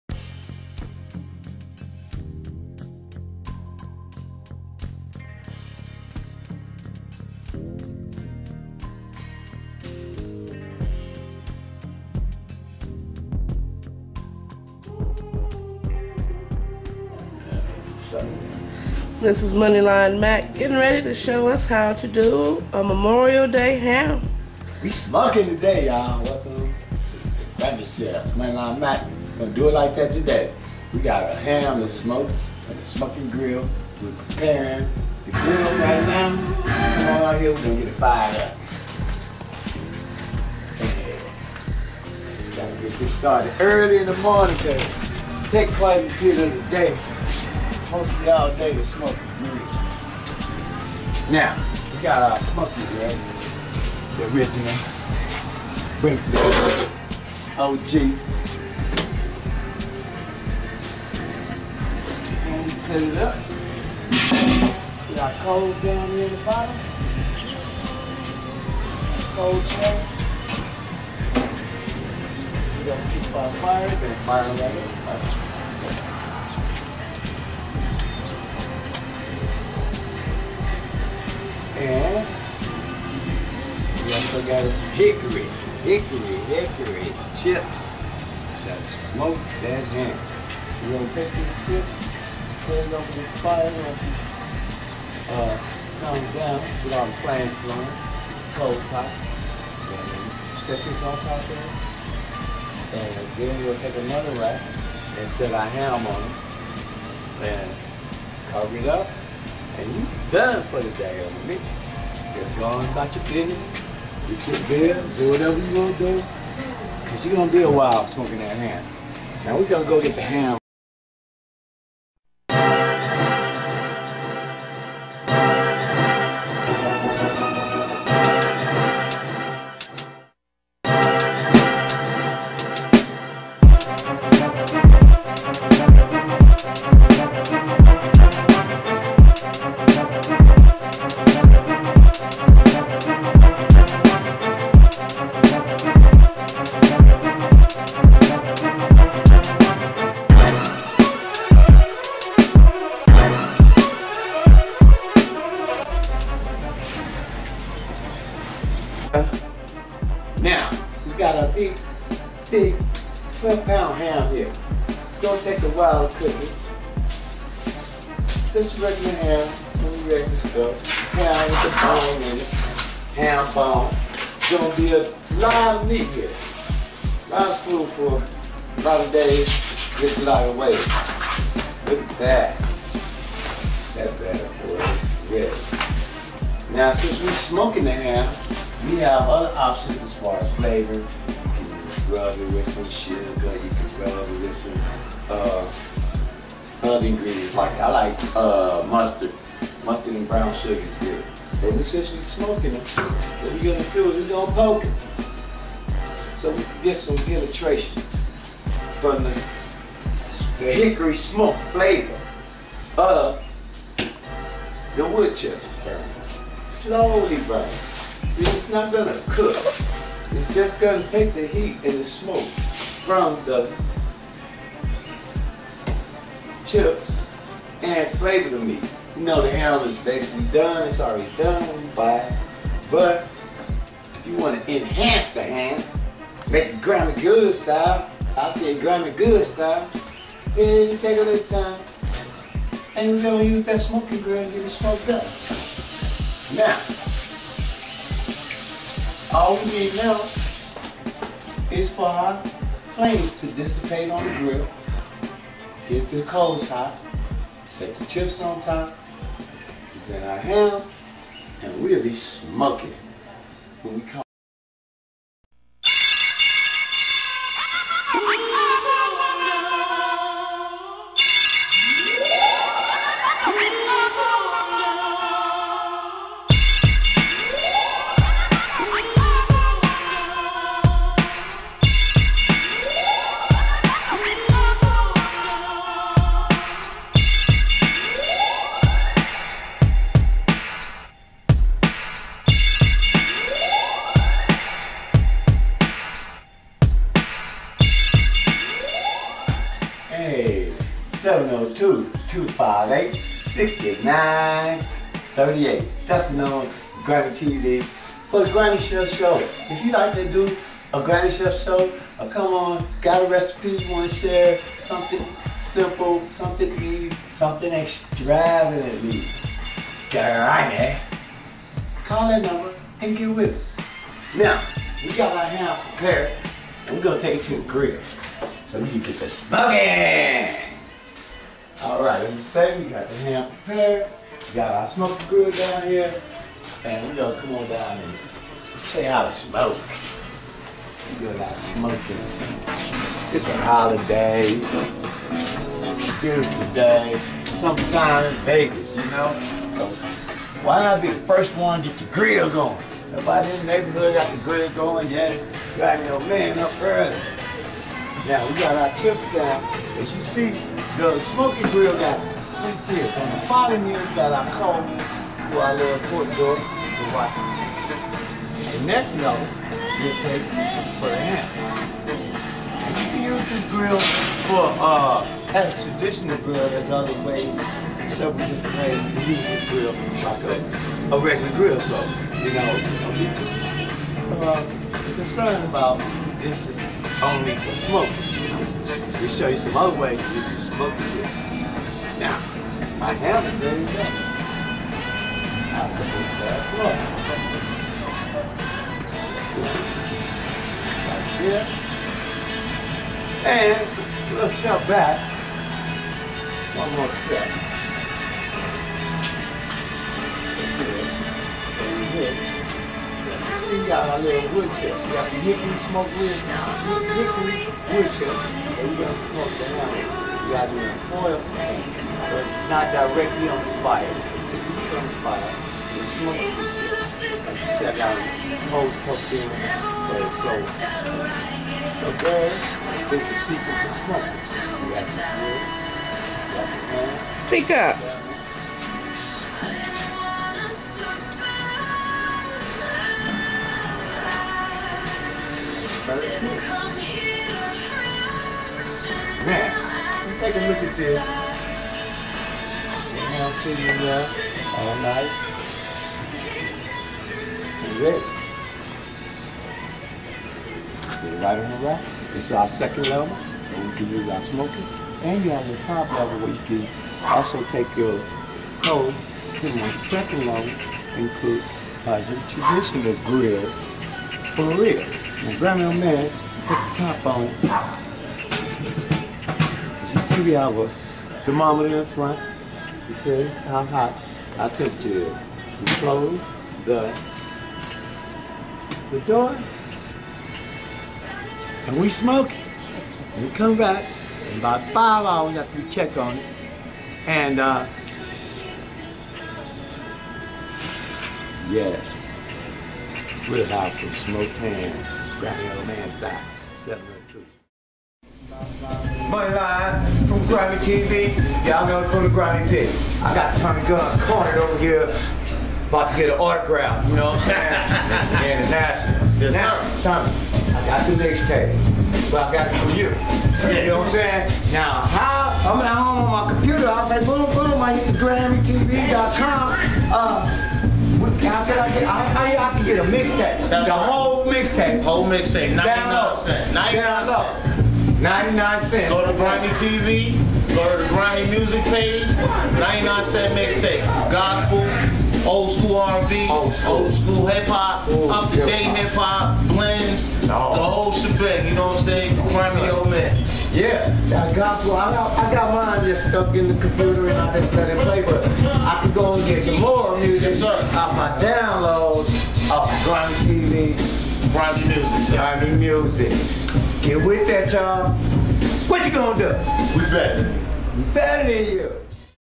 Grimmey Chefs Memorial Day Ham pt.1.mpg
Moneyline Mack is a true Grimmey Chef showing you all how to smoke a ham on Memorial Day